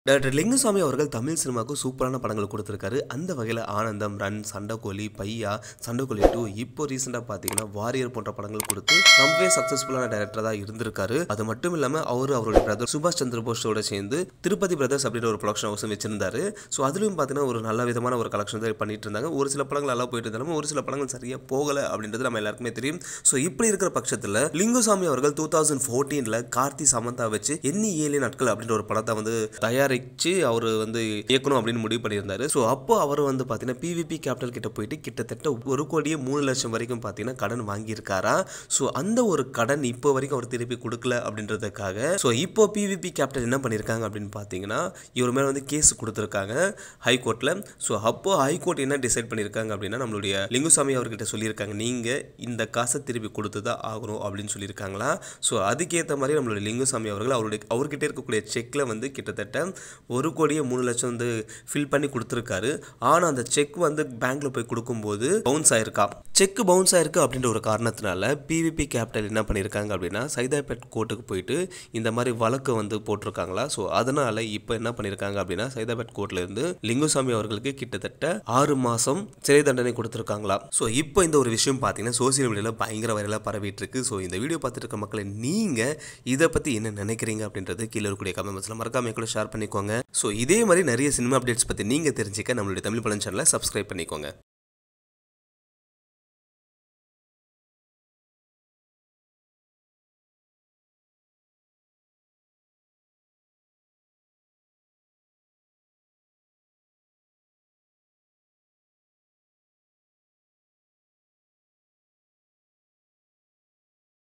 Director Lingusamy Arrested For Financial Crime | லிங்குசாமிக்கு சிறை தண்டனை | Thamizh Padam
Director Lingusamy Arrested For Financial Crime. லிங்குசாமிக்கு சிறை தண்டனை. Director Lingusamy has been sentenced to 6 months imprisonment by the Saidapet court. A case has been filed against the director by a production company as the director did not repay the money he had bought from the production house. Director Lingusamy has reportedly surrendered a cheque quoting the loan money to the production house. But the director hasn’t paid the loan money and the cheque surrendered by him has bounced.